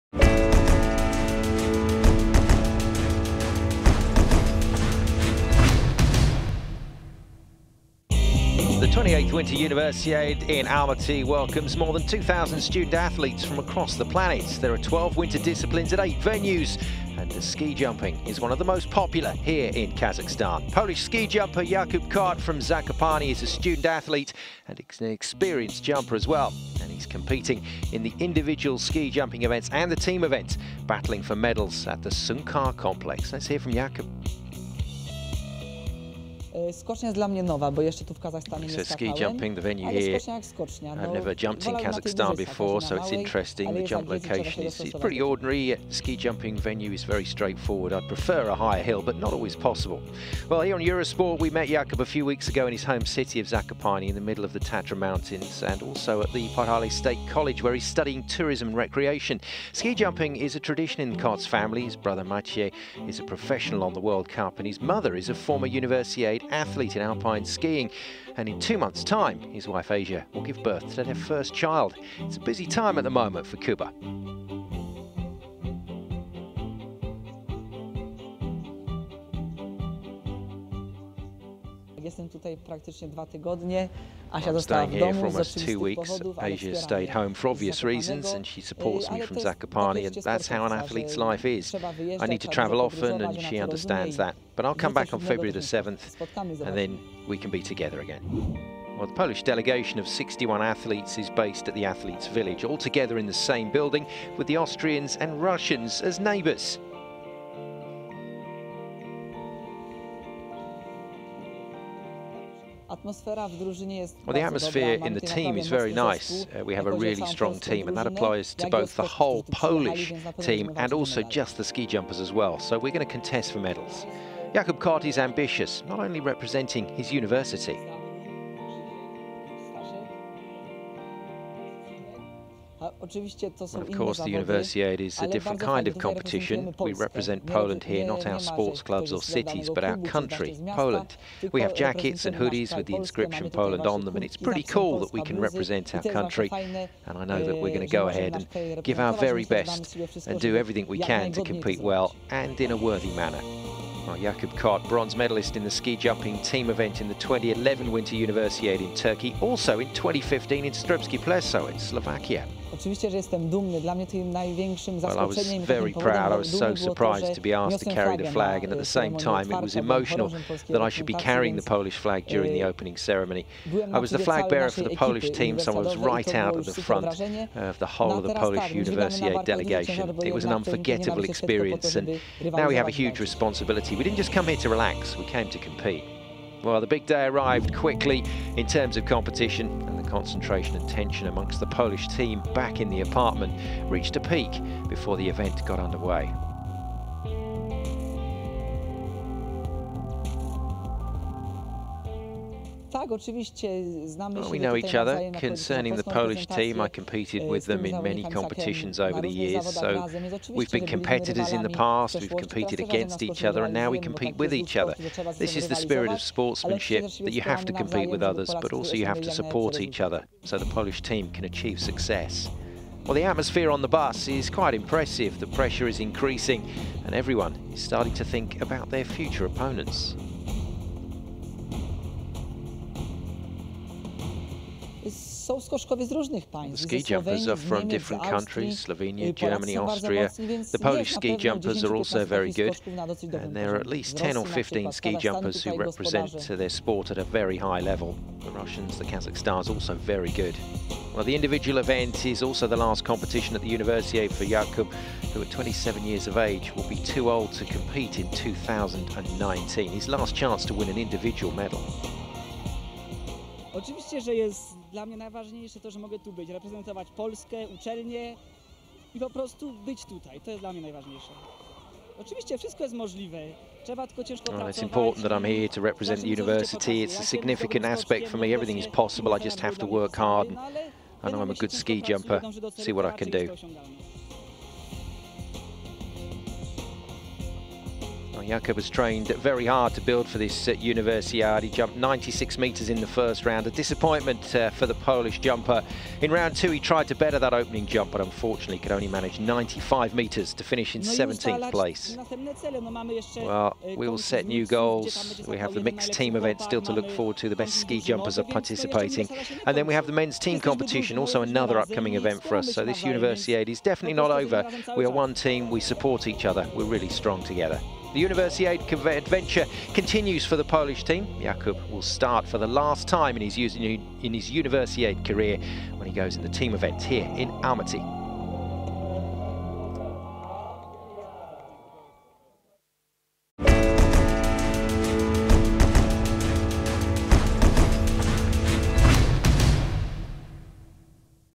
Athlete Story - Jakub KOT (POL) Ski Jumping - 28th Winter Universiade 2017, Almaty, Kazakhstan
Almaty - The Polish Jakub Kot takes part in his third Universiade in Almaty. Discover the course and daily life of this jumper, the flag bearer of Poland.